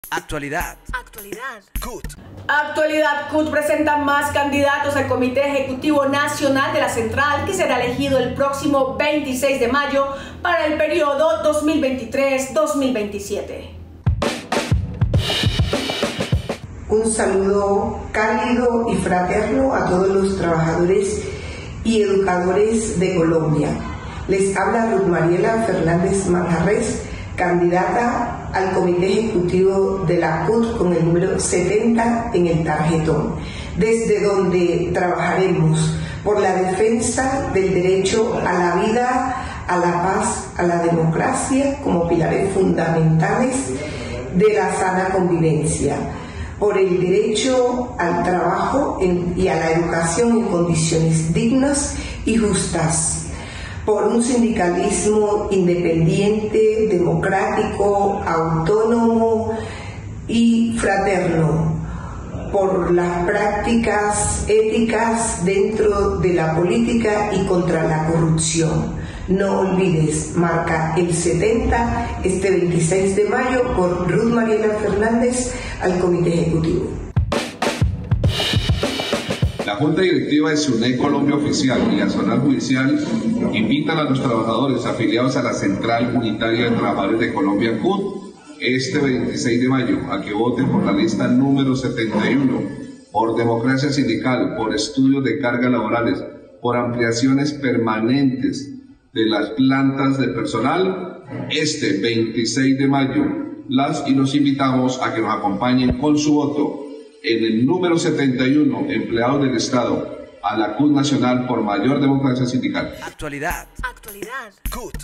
ActualidadCUT - 06 de mayo. Presentación de candidatos a comités ejecutivos de la CUT VIII.
Actualidad CUT presenta más candidatos al Comité Ejecutivo nacional de la Central que será elegido el próximo 26 de mayo para el periodo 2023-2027.